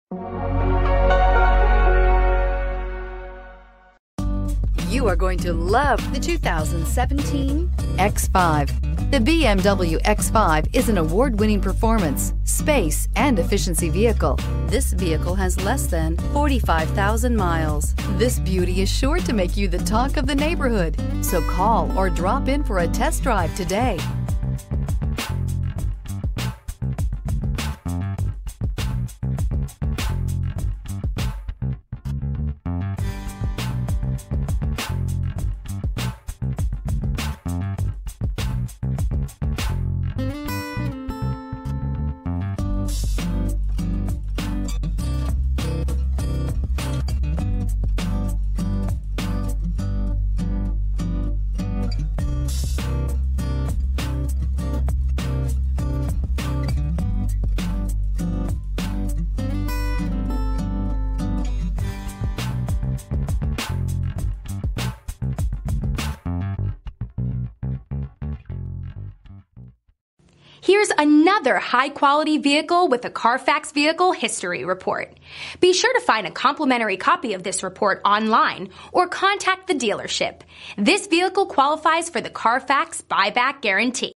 2017 BMW X5 Chattanooga TN AC3223A
2017 BMW X5 sDrive35i

Clean CARFAX. Dark Graphite Metallic 2017 BMW X5 sDrive35i RWD 8-Speed Automatic 3.0L I6 DOHC 24V TwinPower Turbo Black w/Dakota Leather Upholstery.Recent Arrival!